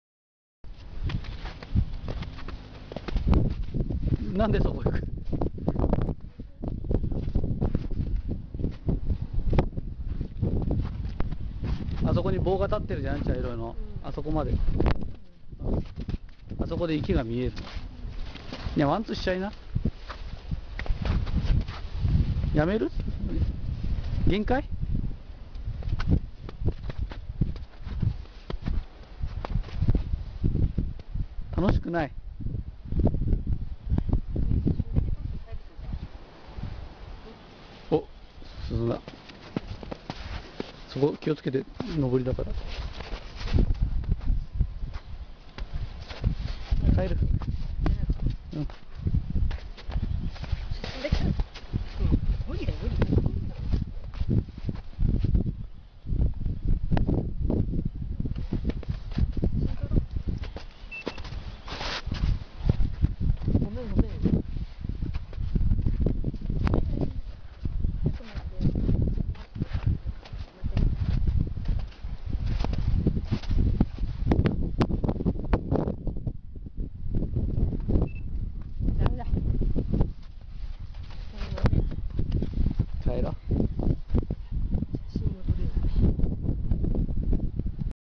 食べていません＠いもり池
つも食べているビデオばかりですが、今日は食べていません(^.^)

うーにーの足腰には、ここは厳しかったみたいです。